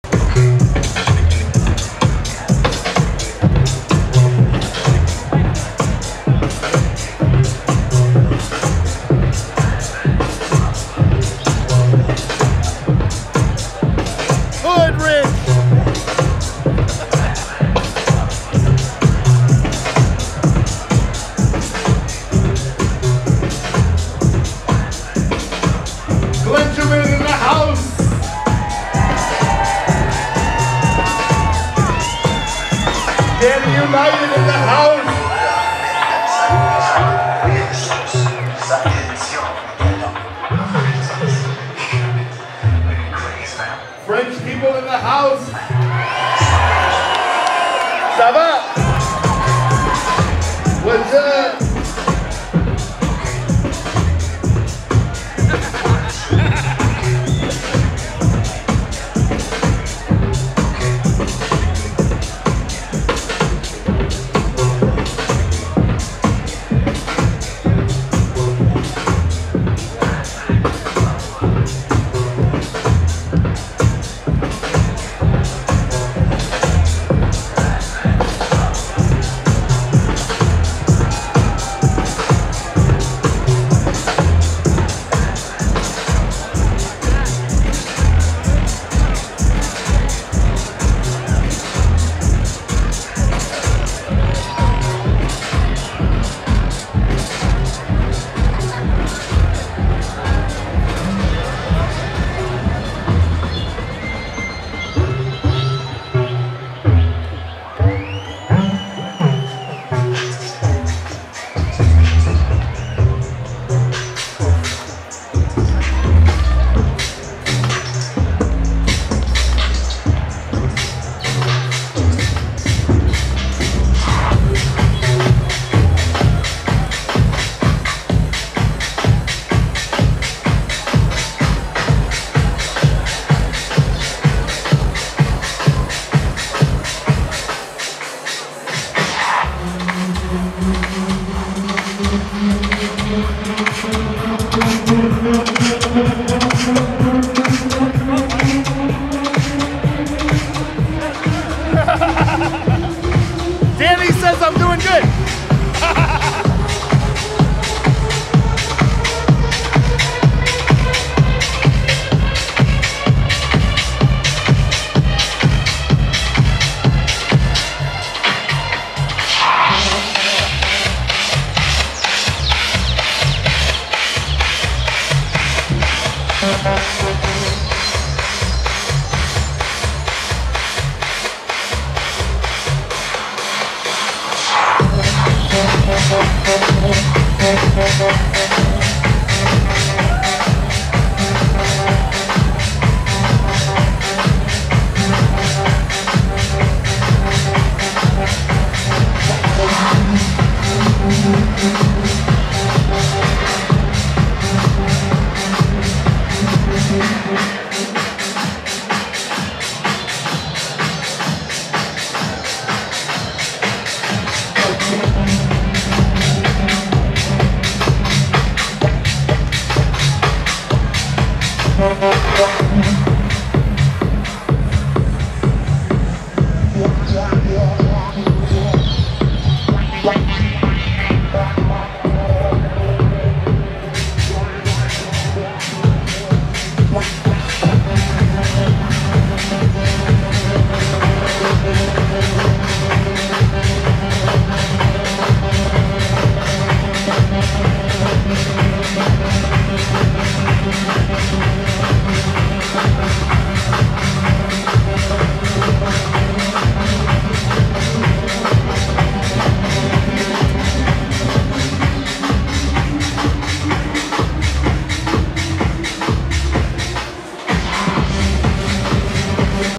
THEE MIKE B - IN THE HOUSE @ OBSERVATORY OC - 6.12.2016
ROLLED DOWN TO CATCH THE MIKE vs MIKE SHOW:

GESAFFELSTEIN & THEE MIKE B... MY FAVORITE MIKES IN THE WORLD...

MUCH LOVE TO THE OBSERVATORY AND ALL THE INSANE OC KIDS THAT BEEN DAY ONE WITH US